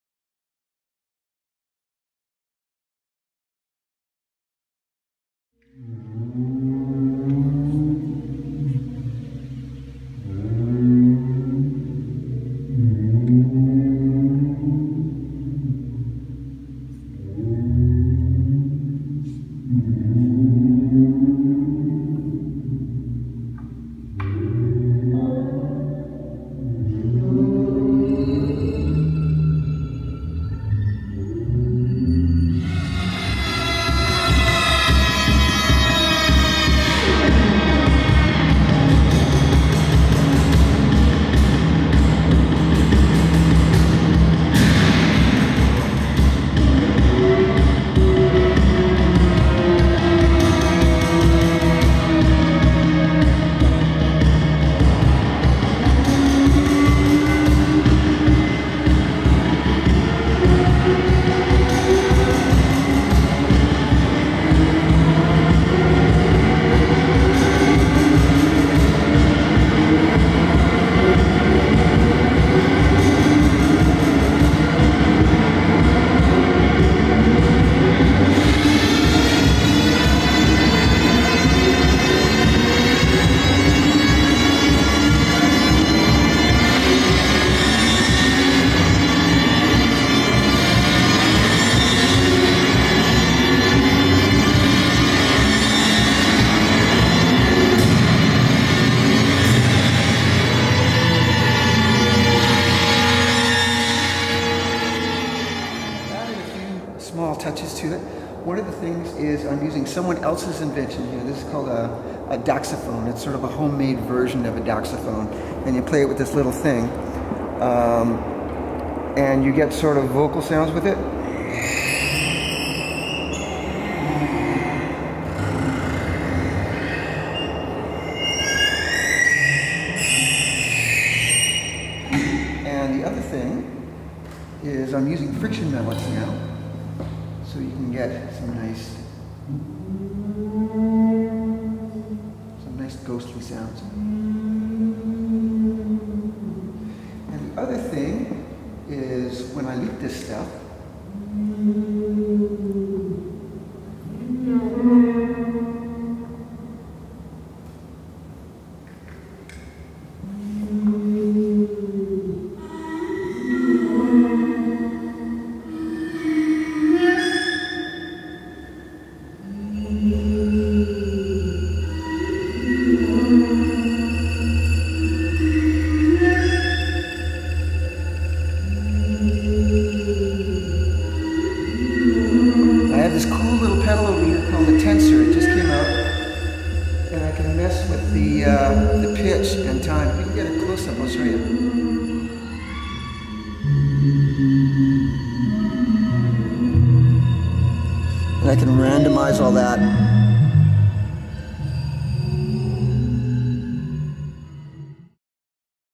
Apprehension Engine: Sound check in Chicago
Doing a sound check with the Apprehension Engine in Chicago for Atlas Obscura's INTO THE VEIL at the Graceland Cemetery, where people wander through the graveyard at night listening to various unusual music and dance performances. Footage shot by Cathy Nosaty.